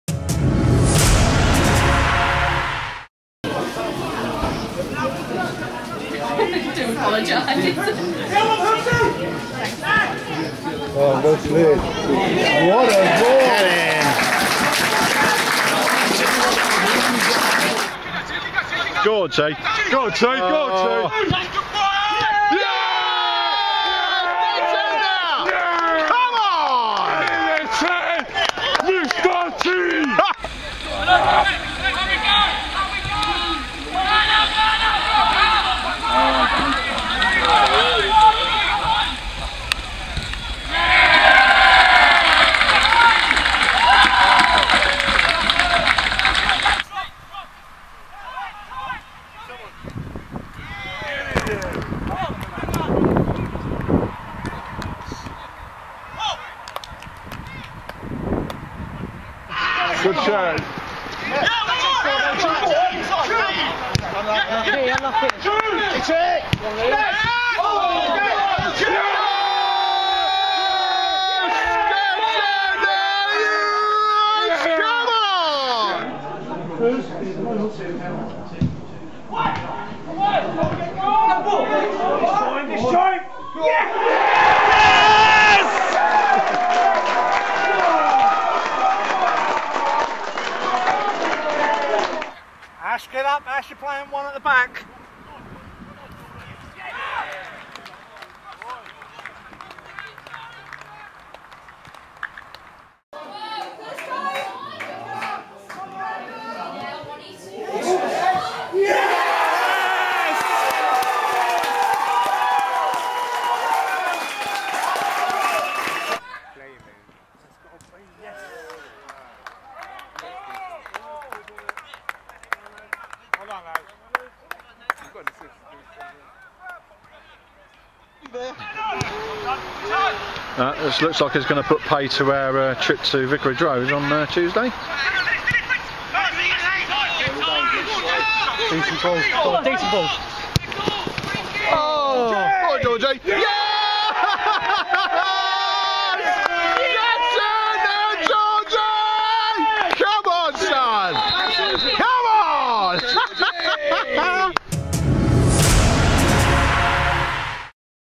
Goals of the Round | The FA Cup 2012 Preliminary Round
It may only be August but The FA Cup rarely sleeps and clubs up and down the country are already on the road to Wembley. FATV is here with the best goals from this weekends preliminary round.